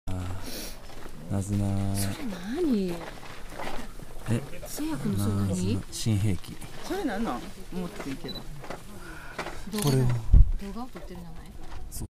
漆 七草粥を食べよう⑦
ナズナ、山グループが採取してくれました。まだ早かったみたい。田んぼの畦には見つからなかった。